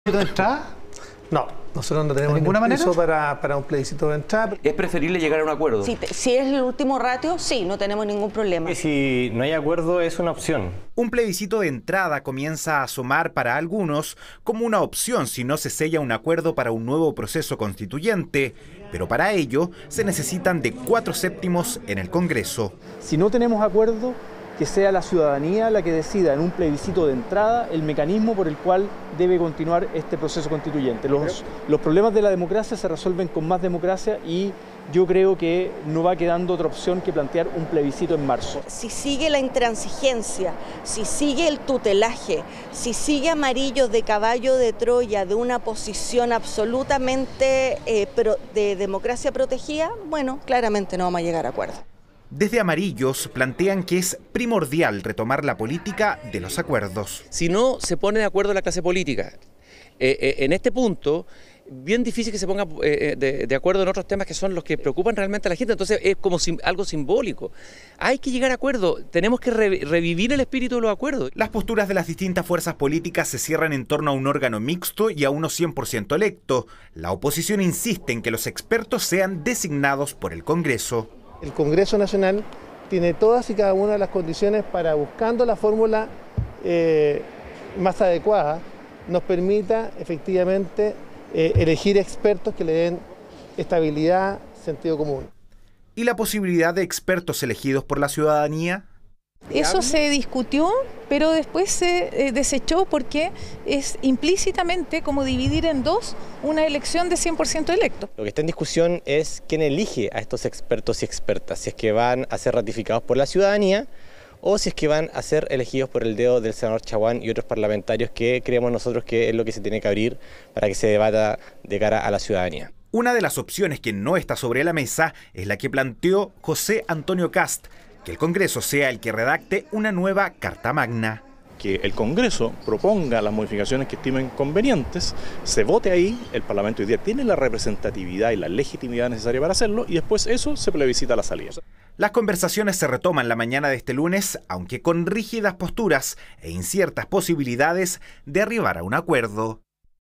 Ante fracaso diálogo constituyente: Oficialismo abre la puerta a un plebiscito de entrada
Ante las fracasadas negociaciones para un nuevo proceso constituyente, el oficialismo abrió la puerta a un plebiscito de entrada en caso de no concretarse un acuerdo entre las distintas fuerzas políticas. Este lunes se retoma el diálogo.